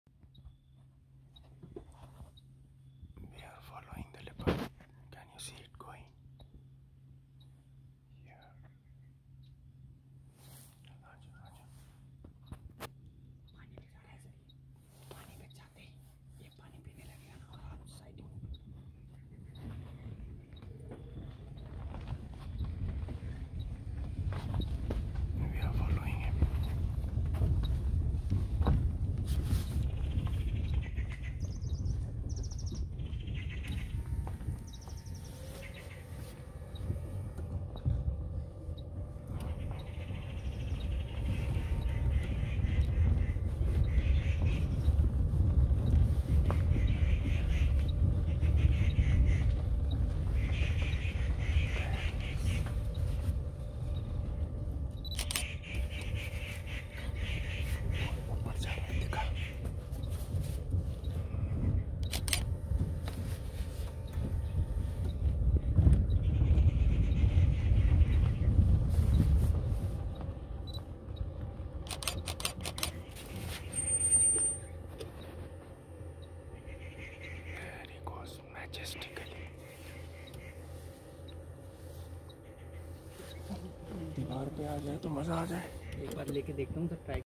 Leopard in Jaipur by Ghumakkad Harsh
Leopard sighting in the city of Jaipur? Yes, take the Safari managed by the Forest Department of Rajasthan India.